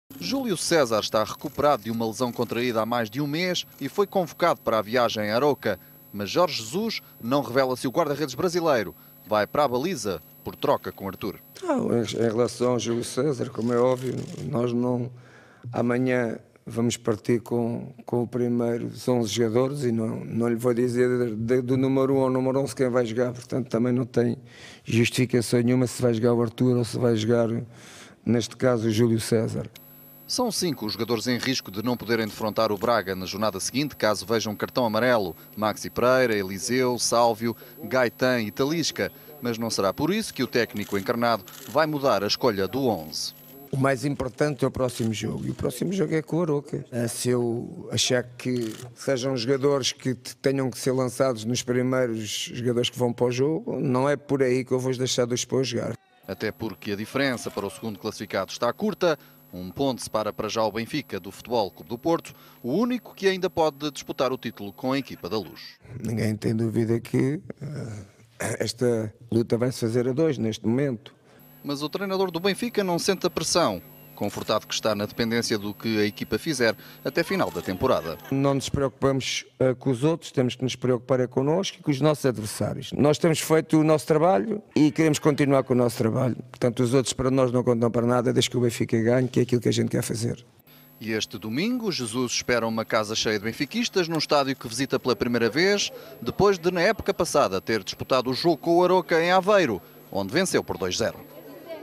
Jesus afirma que não se sente pressionado pela vitória do FC Porto
Jorge Jesus diz que o Benfica não vai jogar pressionado pela vitória do FC Porto. Os encarnados jogam amanhã em Arouca. .